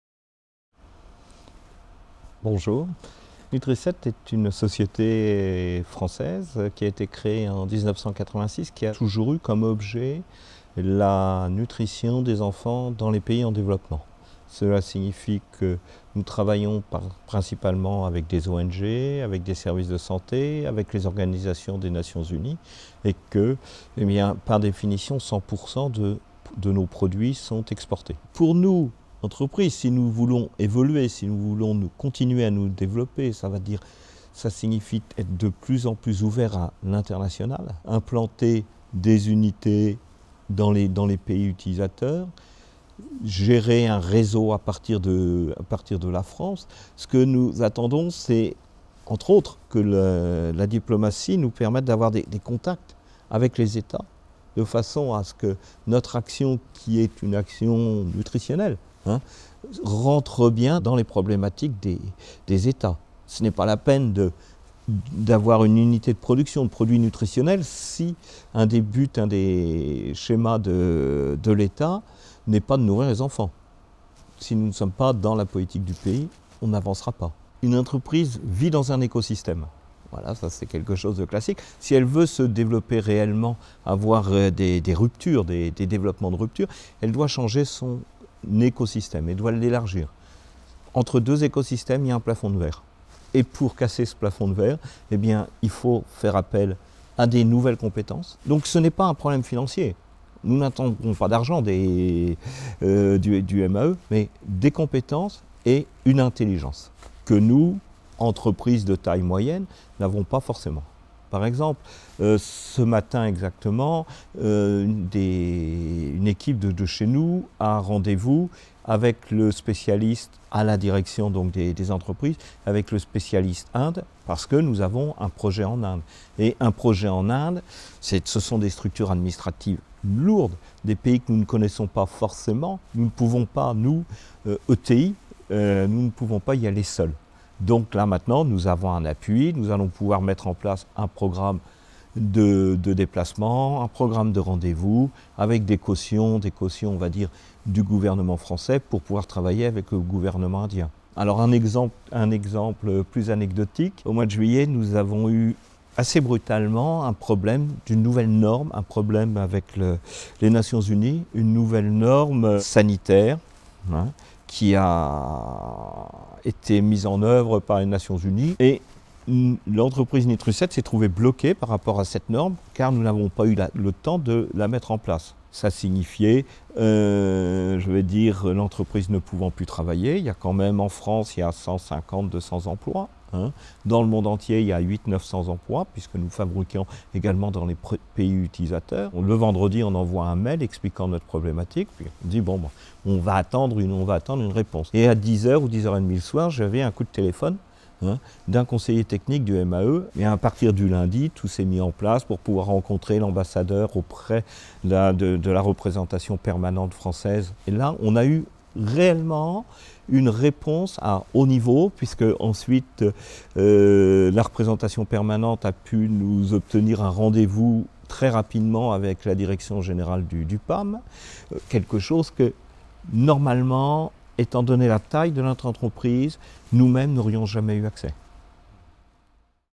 Diplomatie économique : Interview de Michel Lescane (04.04.2013)
Rencontrediplomatie et entreprise : interview de Michel Lescane, président et fondateur de Nutriset (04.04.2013).

Laurent Fabius a fait de la diplomatie économique l'une de ses priorités. C'est à ce titre que le Quai d'Orsay a ouvert ses portes aux entreprises à l'occasion des Rencontres Quai d'Orsay - Entreprises le mardi 9 avril.

Instragram : @francediplo